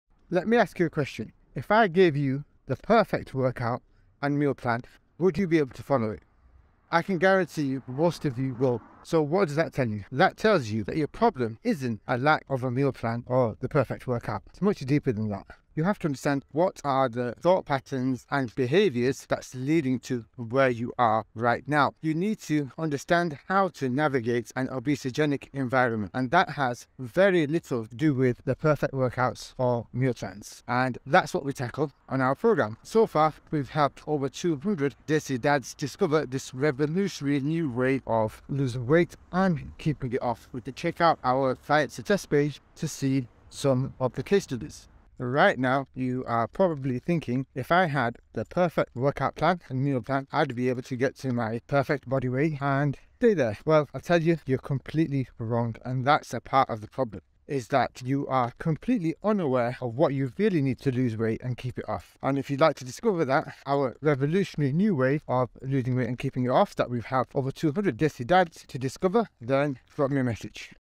Are you just missing the perfect workout plan?
You might think if you just had the perfect workout and meal plan you'd be lean for life.

But is that assumption correct,  watch this video to find out.

Once you know what the real problem is you'll be well on your way to tackling it